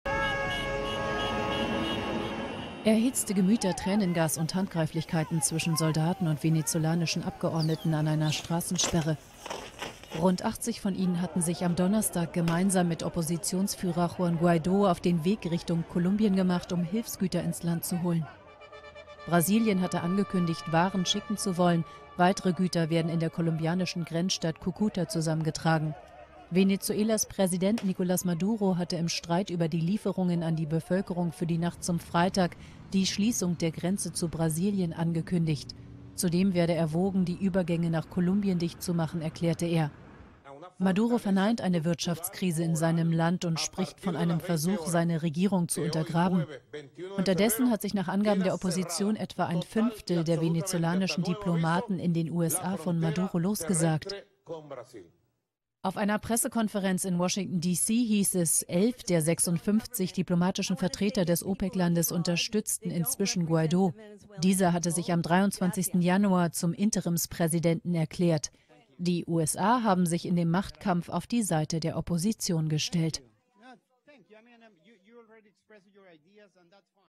Streit um Hilfslieferungen nach Venezuela spitzt sich zu
Die venezolanische Opposition unter der Führung von Juan Guaido hat angekündigt, am Samstag Hilfslieferungen aus Kolumbien ins Land zu holen. Präsident Nicolas Maduro reagiert mit weiteren Grenzschließungen.